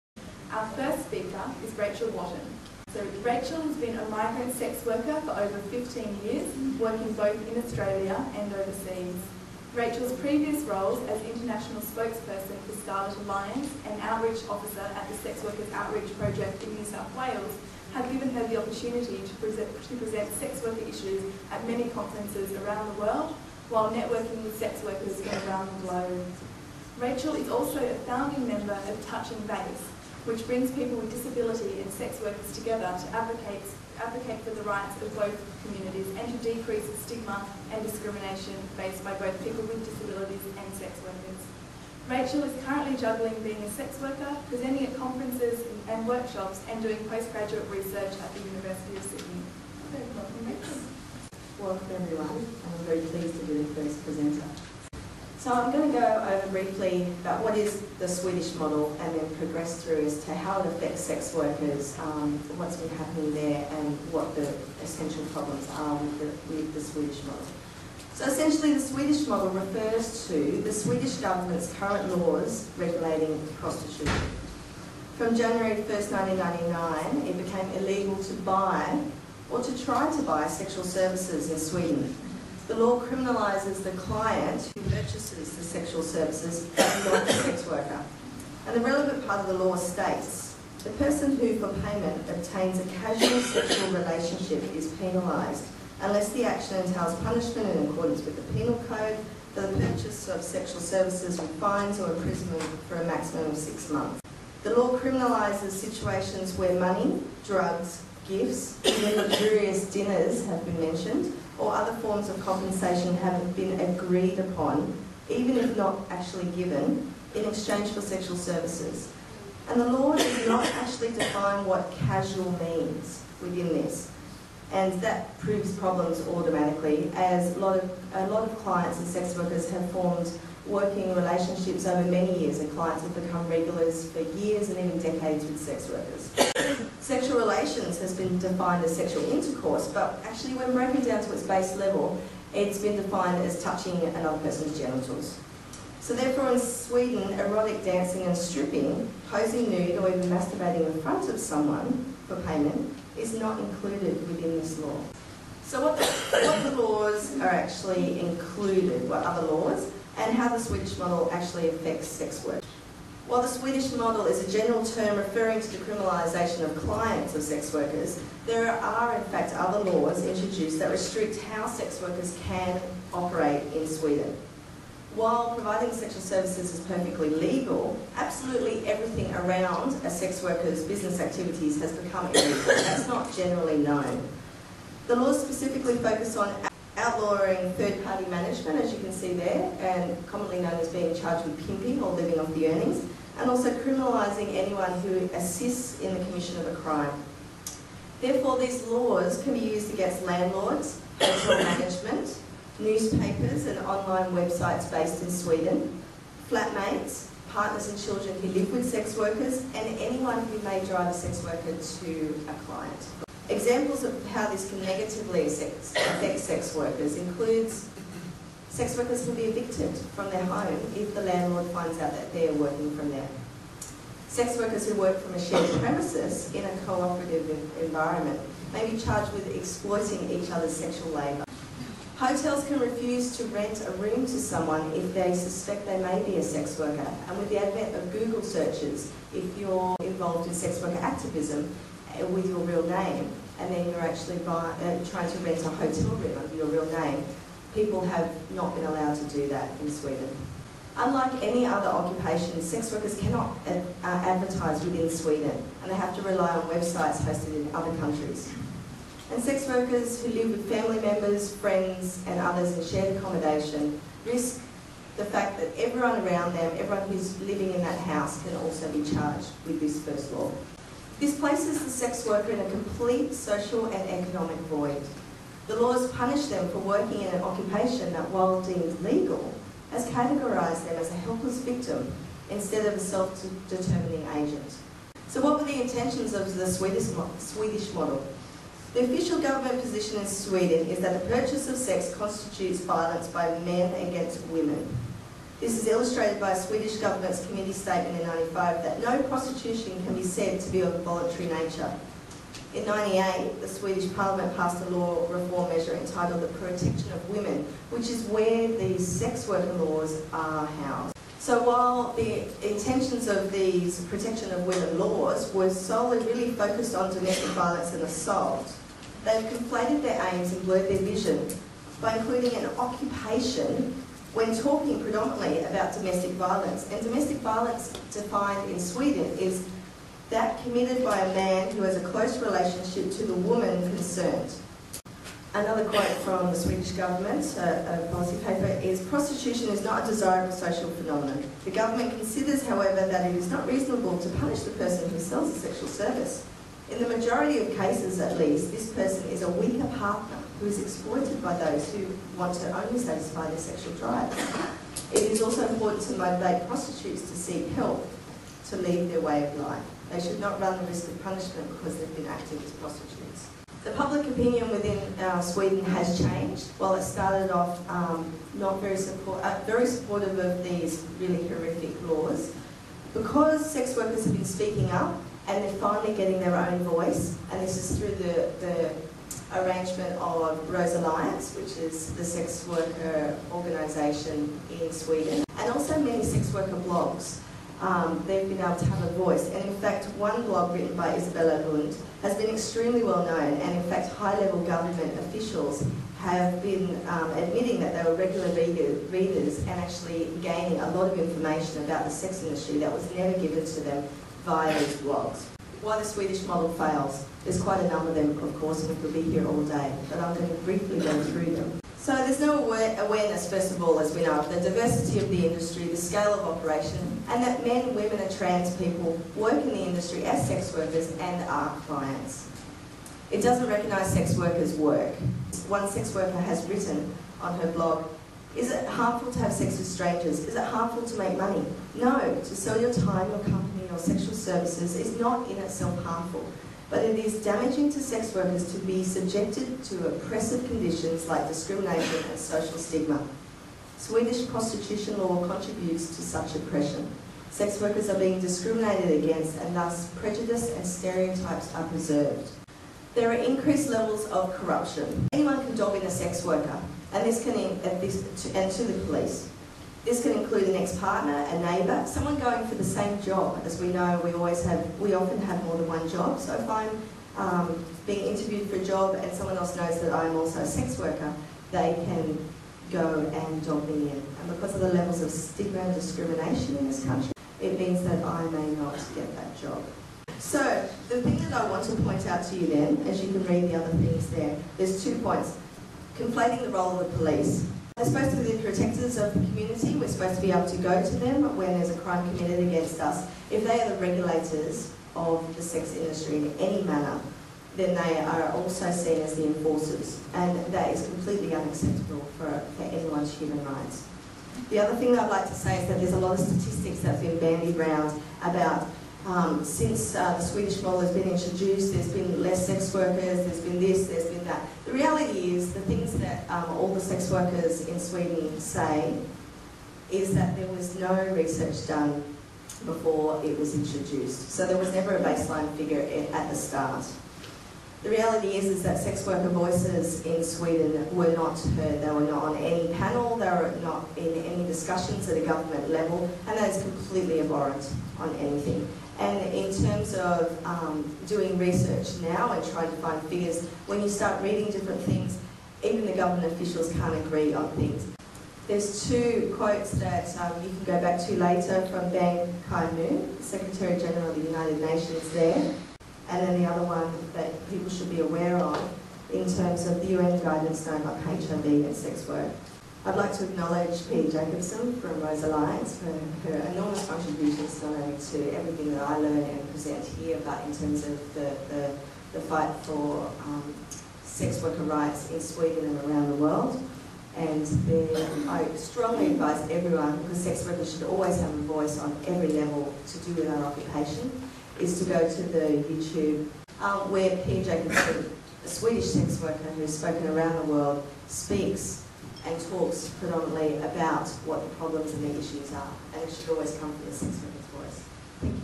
Part 1 Scarlet Alliance National Symposium 2009: Rachel Wotton on the Swedish Model
Rachel Wotton, at the 2009 Scarlet Alliance National Symposium, speaking on the Swedish Model and why it fails sex workers.